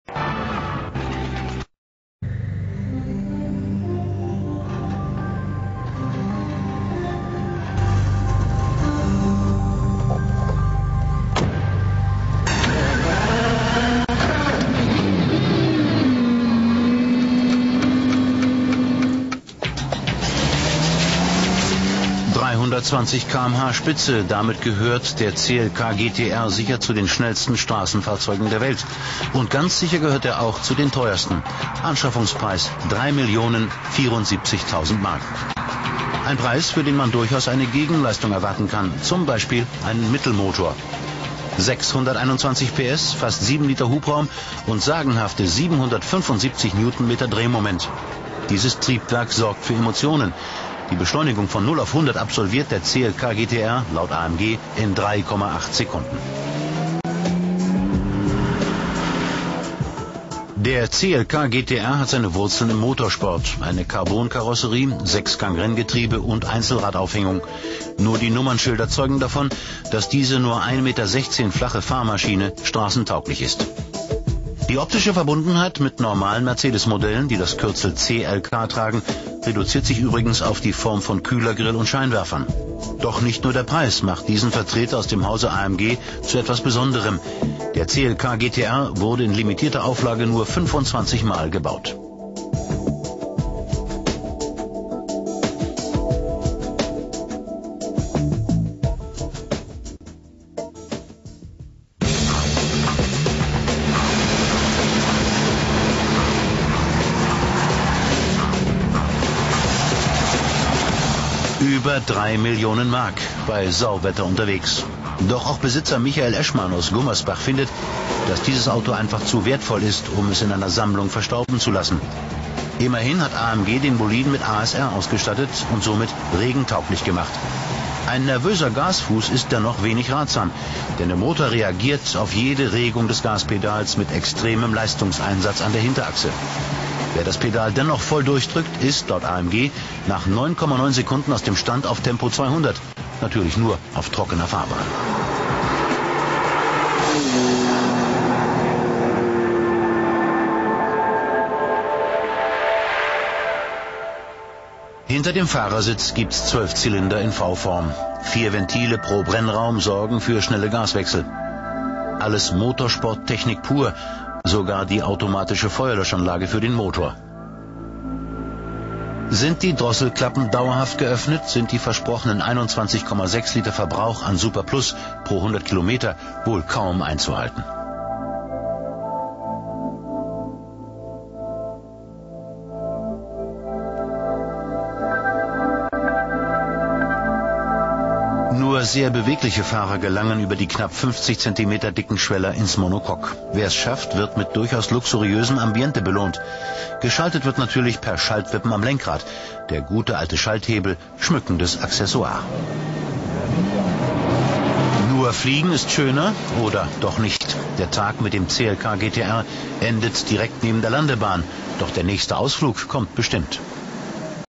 CLK GTR
this is the greatest production mercedes ever made! with its 800,000 price tag it aint nothing to be sneezed at!